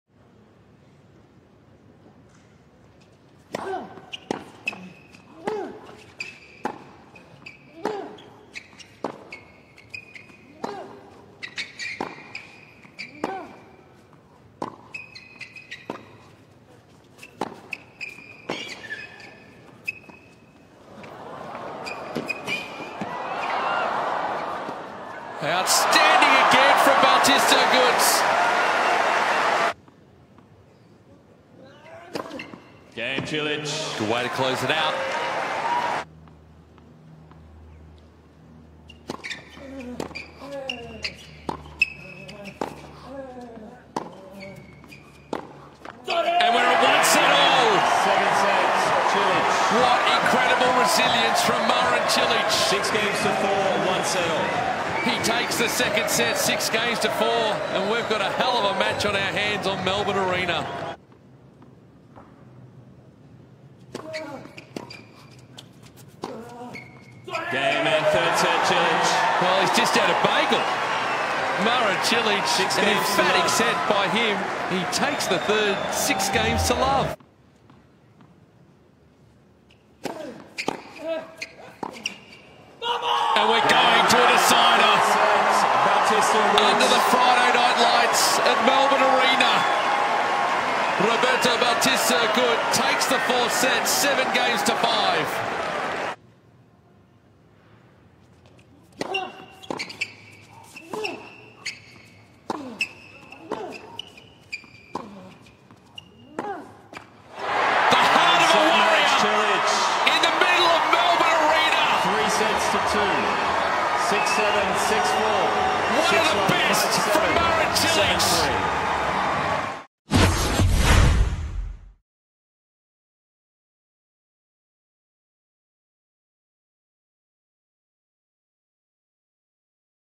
Marin Cilic vs Roberto Bautista Agut - Match Highlights (3R) | Australian Open 2020
Watch the match highlights from Marin Cilic vs Roberto Bautista Agut in the third round of the Australian Open 2020.